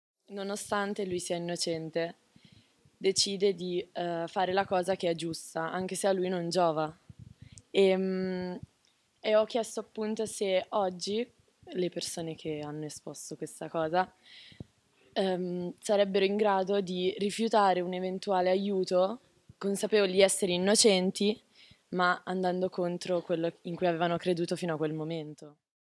miniinterviste - Studenti in sala - Il Critone - Centro Asteria
°°° FARE LA COSA GIUSTA CONTRO IL PROPRIO INTERESSE
E' possibile accettare una decisione altrui perchè "giusta" nonostante ci metta in una condizione svantaggiosa?
Una studentessa durante il dibattito esprime i dubbi e le domande suscitate dalla visione dello spettacolo IlCritone.